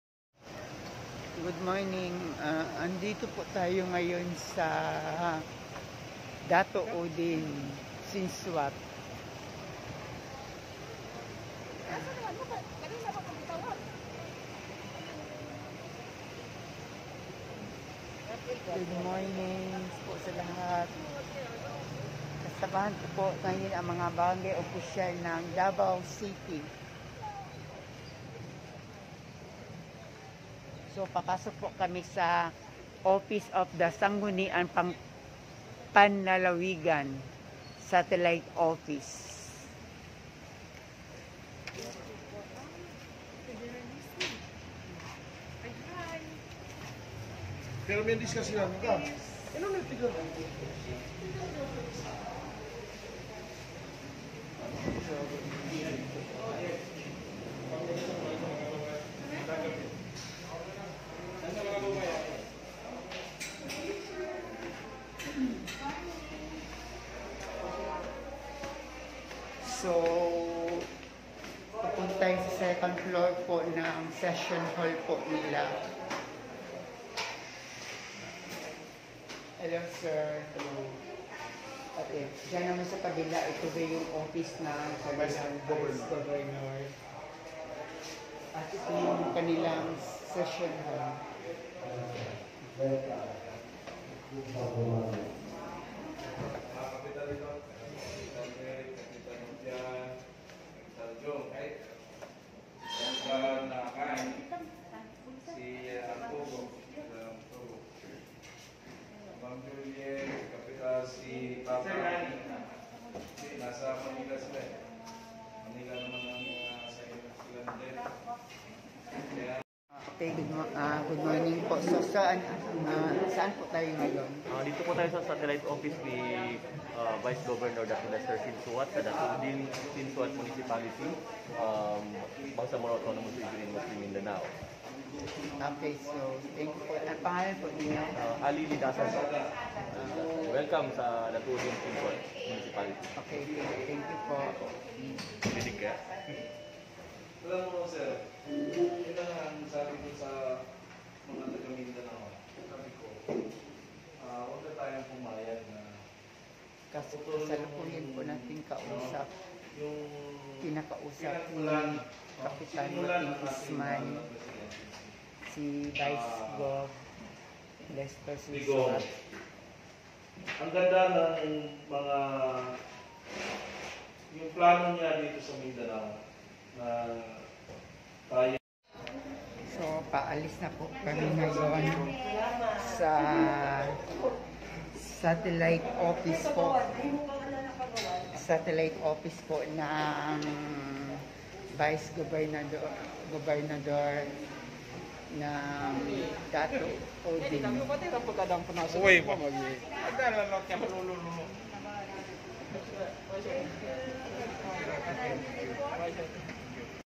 Run Sara Run in Datu Odin,Maguindanao SaraAllPhilippines2022 👊👊👊🇵🇭♥️
Binisita ng Team Duterte for federalism Movement Ang Lalawigan ng Datu Odin,Maguindanao
Headed by Kap Wating Usman